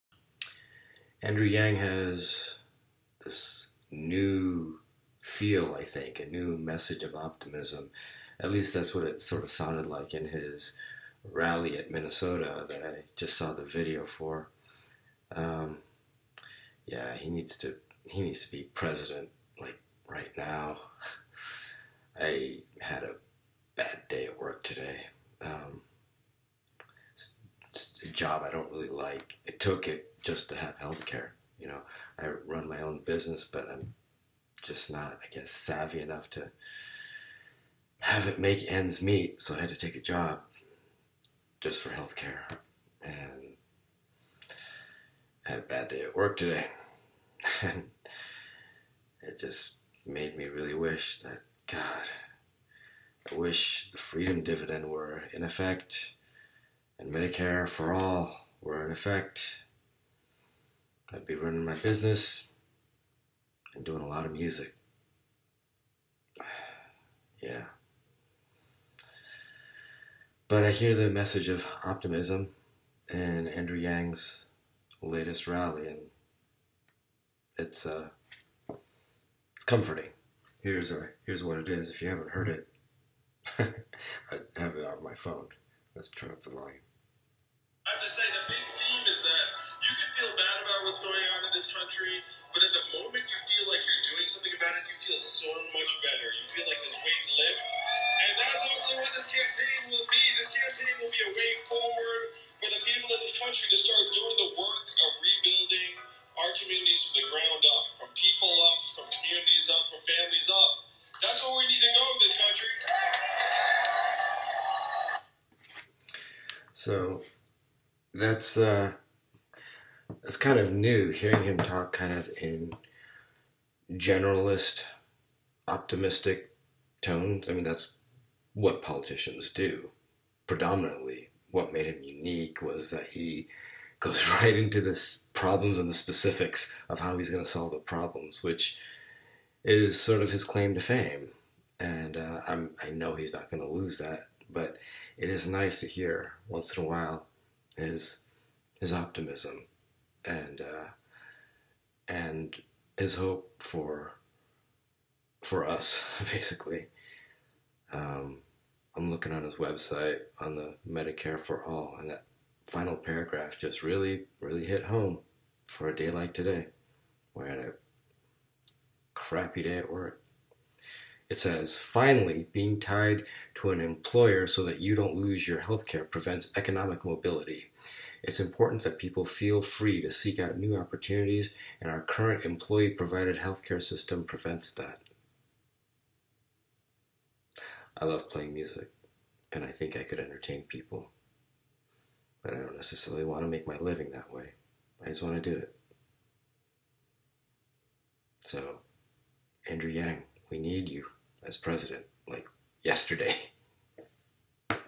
Andrew Yang's New Message of Optimism
Had a bad day at work today. Hearing him say this helped.